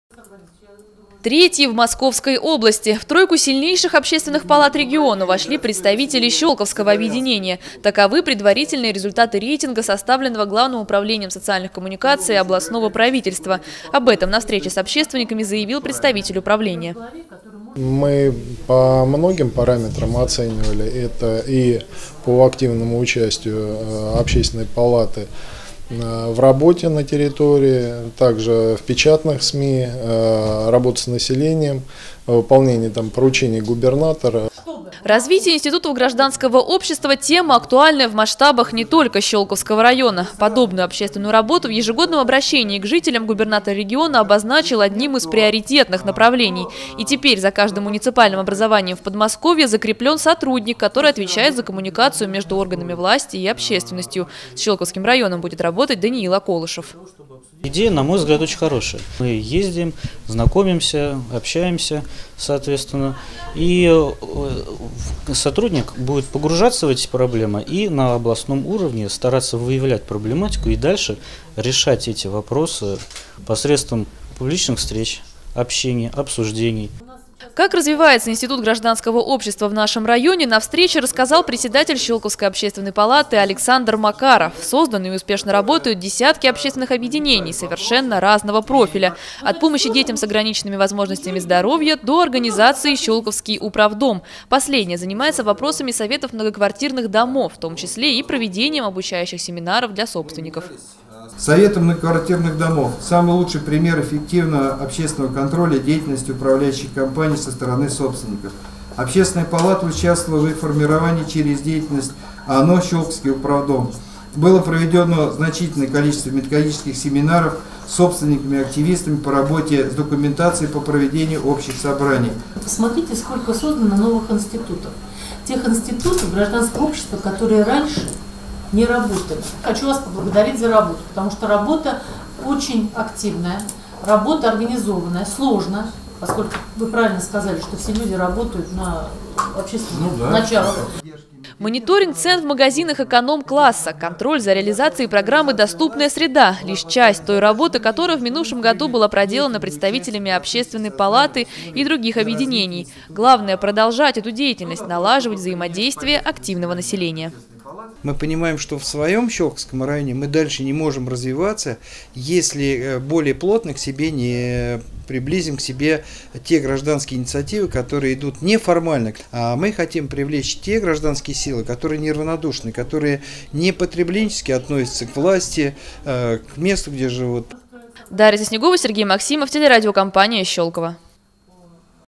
Развитие институтов гражданского общества в 2015-м году
О развитии институтов гражданского общества в 2015-м году говорили на заседании Общественной палаты района. О том, насколько активно проявили себя представители общественности, рассказал председатель объединения Александр Макаров